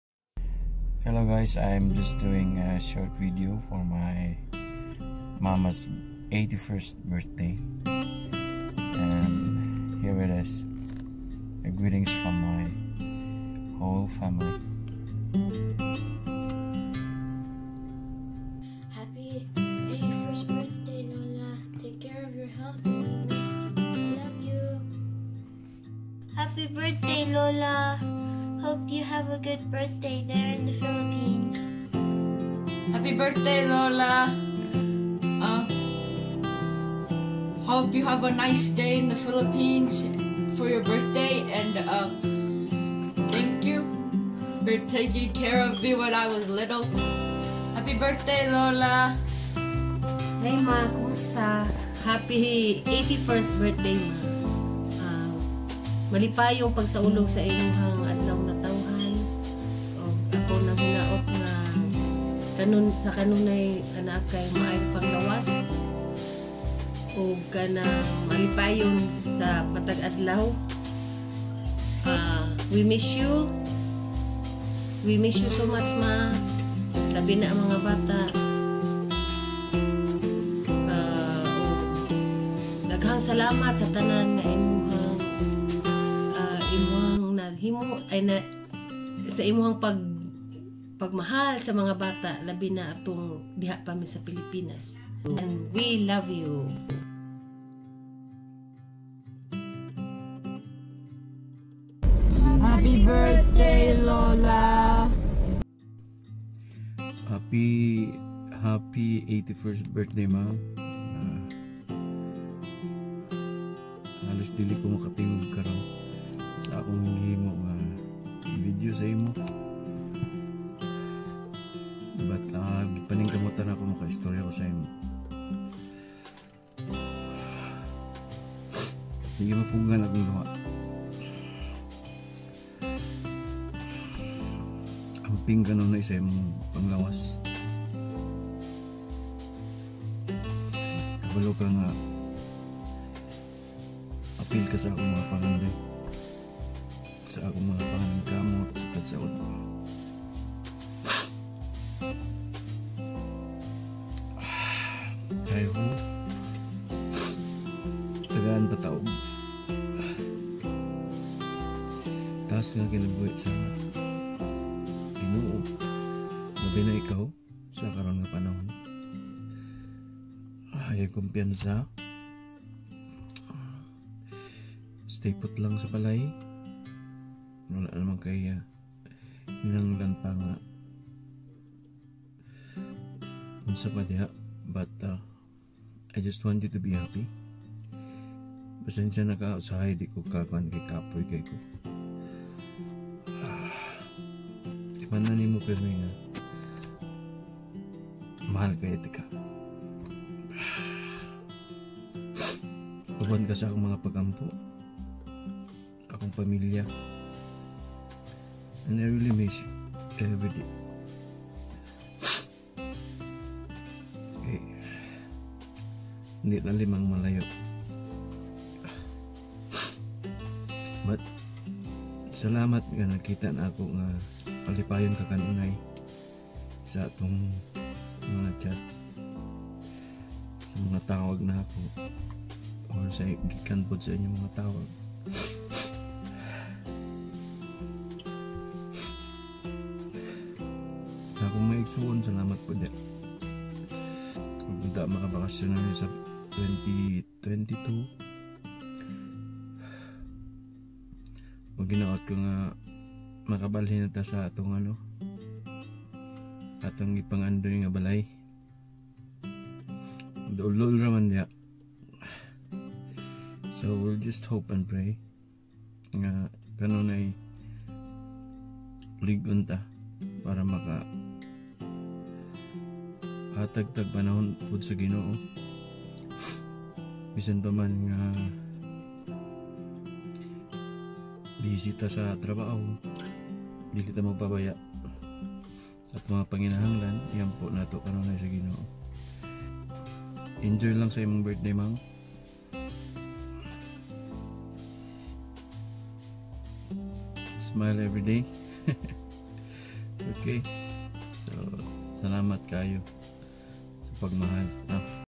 81st Birthday greetings for my beloved mother.
Making this short video takes a lot of hearts. I mean am just thankful to the Lord for giving my mother a long life, not just that but blessing her so healthy even at 81 year old still alive and kicking.